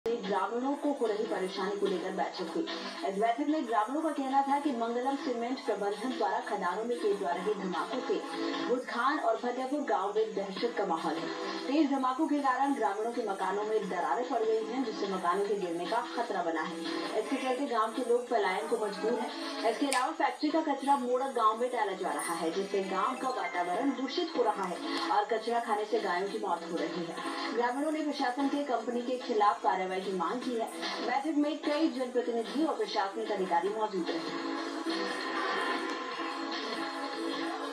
मंगलम सीमेंट का अमंगल Etv news
रामगंजमंडी
मंगलम कर रही गांवो में अमंगल ,,,, Etv News
Video from Sabir Khan, Etv